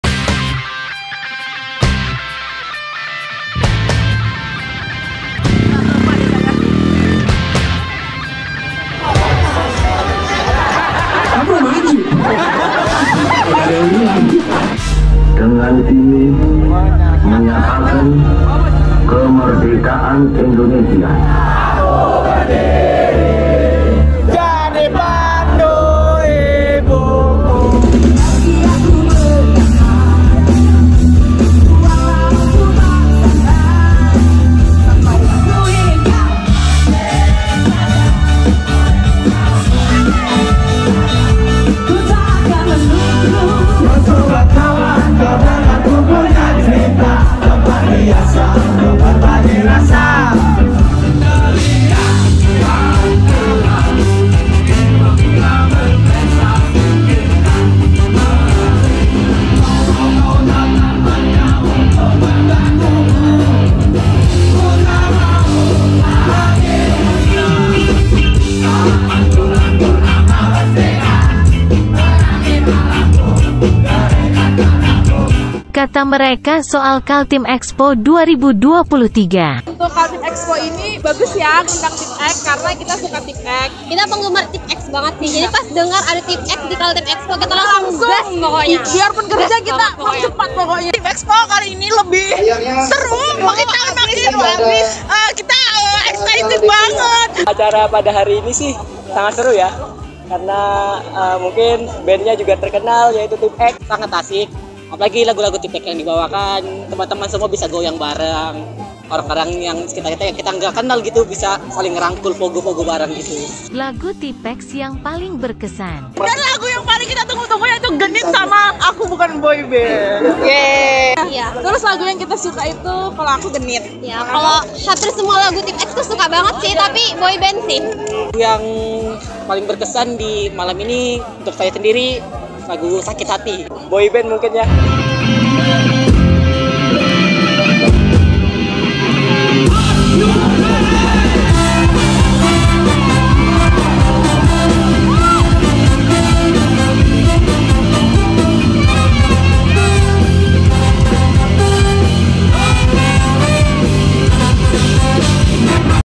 Tipe-X Bikin Ribuan Hadirin Konser Kemerdekaan Kaltim Expo Jingkrak-Jingkrak | Kaltim Faktual
Tipe-X sukses bikin penonton Konser Kemerdekaan Kaltim Expo jingkrak-jingkrak. Semua pada loncat bareng di ‘Pesta Ska’ Samarinda, sambil basah-basahan mandi hujan ala pemadam.

Baca selengkapnya di | Kaltim Faktual